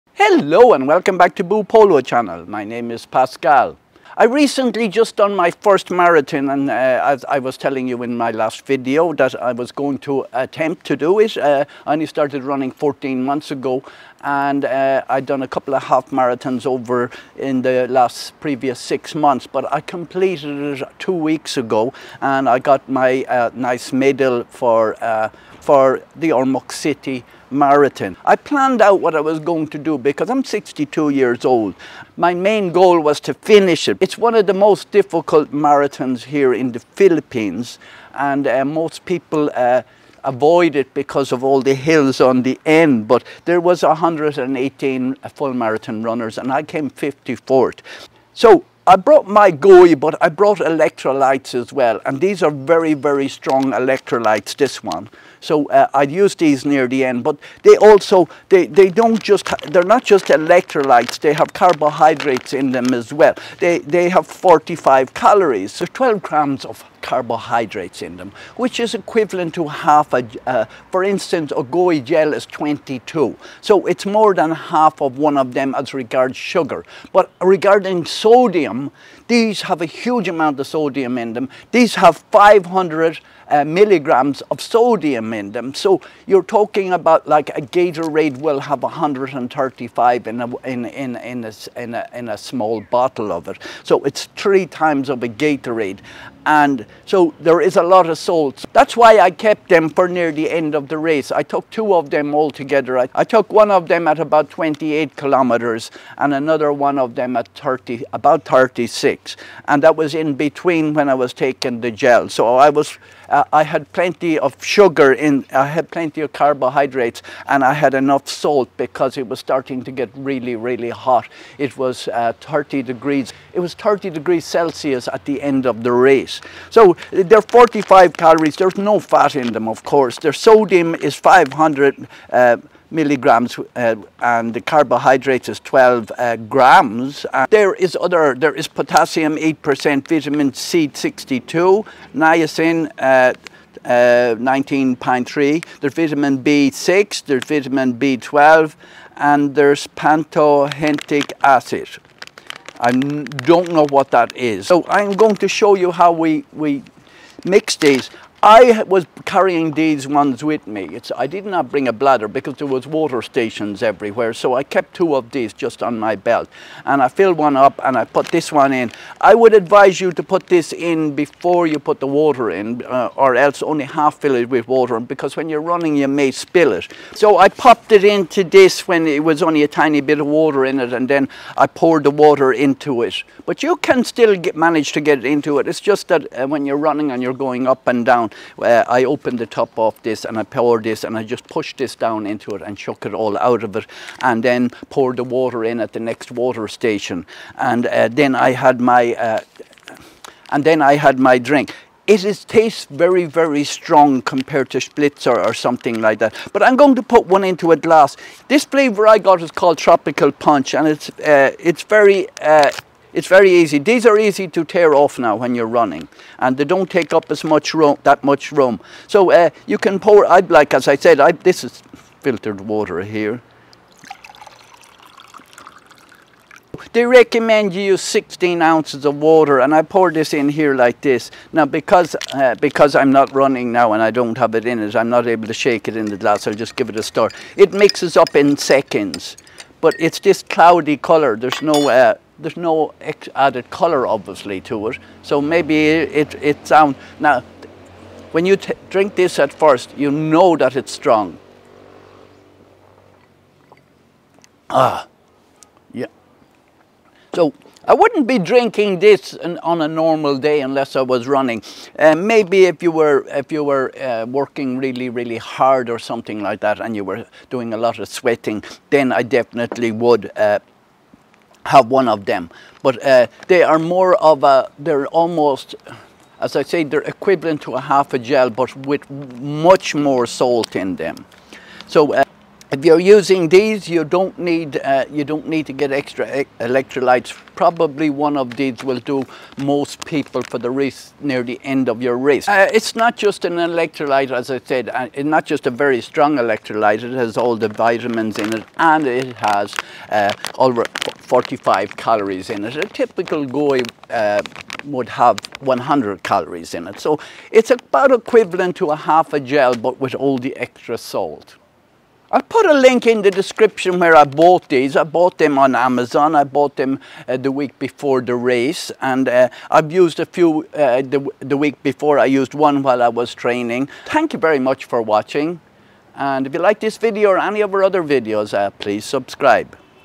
LIQUID IV High Sodium Hydration Multiplier Electrolytes - Review
In this video, I review these sachets of electrolytes by LIQUID I.V.

We use Geniuslink deep links for your convenience
Boopolo, Irish Chippy, and growNglow